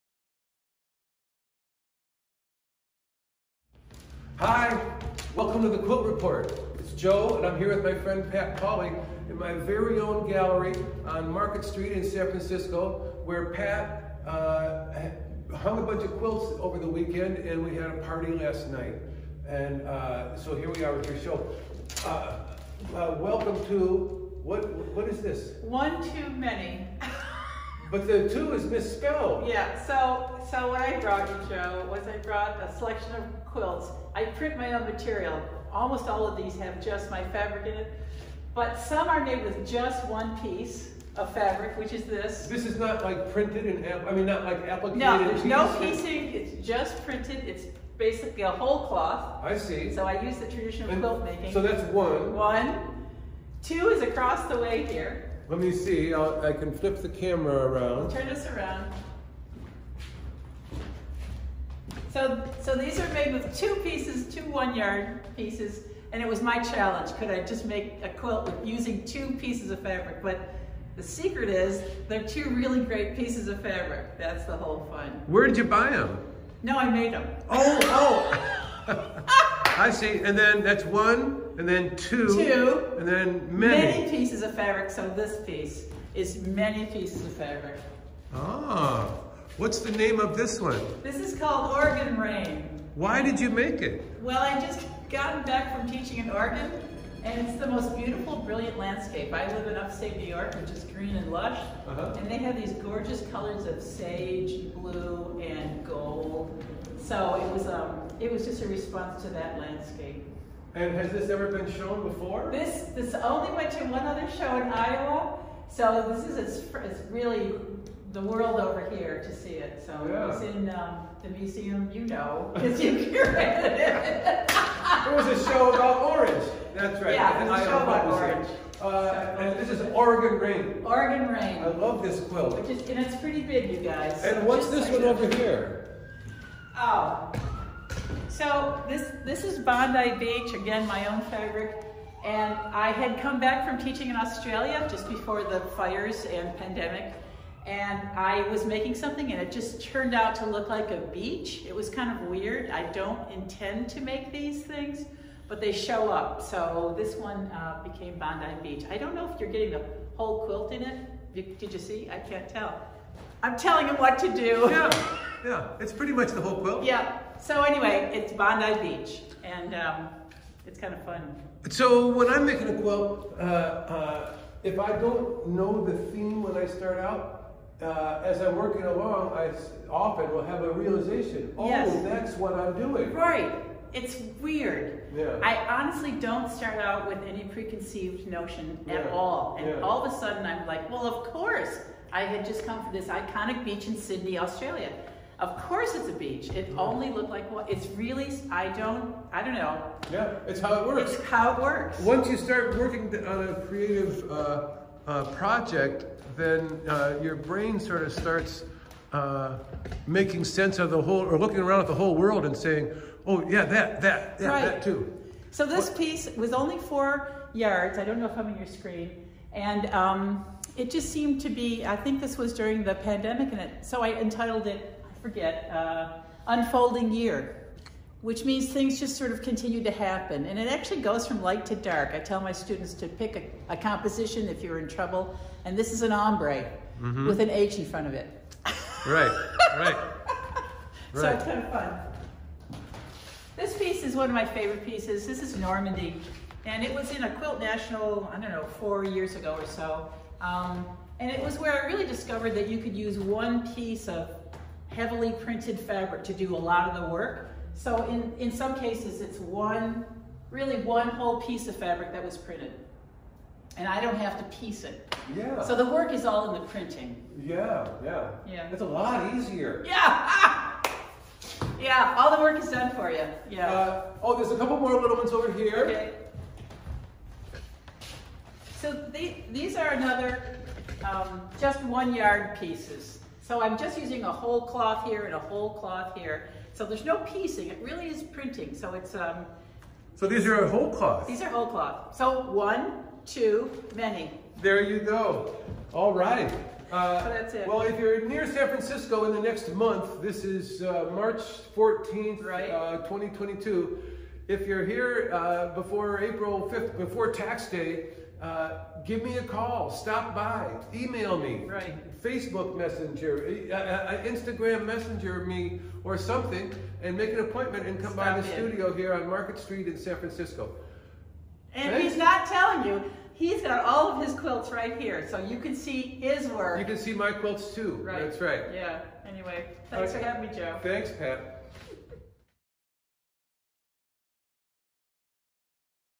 The Quilt Report 94
A video tour of Pat Pauly's exhibition at my gallery, "One Two Many."